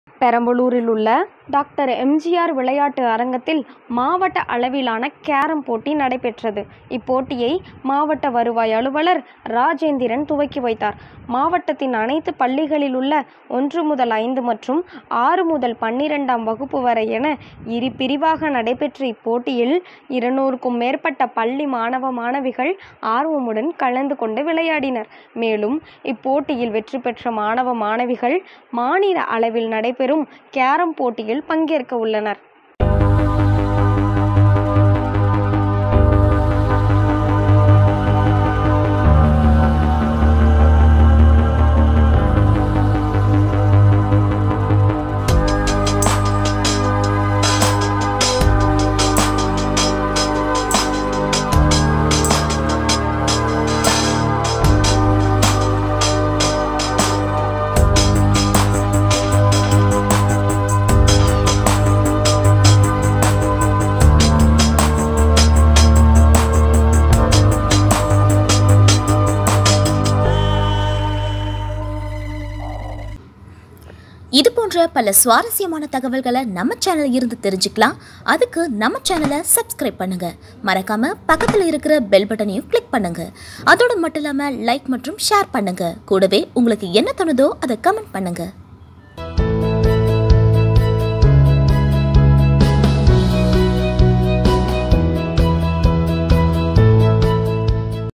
மாணவர்களில் கலக்கப் போவது யாரு...? | Game for Students
பெரம்பலூரில் உள்ள  டாக்டர் எம்ஜிஆர் விளையாட்டு அரங்கத்தில்   மாவட்ட அளவிலான கேரம் போட்டி நடைபெற்றது. இப்போட்டியை மாவட்ட வருவாய் அலுவலர் ராஜேந்திரன் துவக்கி வைத்தார் .
மாவட்டத்தின் அனைத்து பள்ளிகளில்  உள்ள ஒன்று முதல் ஐந்து மற்றும்  6 முதல் 12 ஆம் வகுப்பு வரை  என இரு பிரிவாக நடைபெற்ற இப்போட்டியில் 200 க்கும் மேற்ப்பட்ட  பள்ளி மாணவ மாணவிகள் ஆர்வமுடன்  கலந்து கொண்டு விளையாடினர். மேலும் இப்போட்டியில் வெற்றி பெற்ற மாணவ மாணவிகள் மாநில அளவில் நடைபெறும் கேரம் போட்டியில் பங்கேற்க உள்ளனர்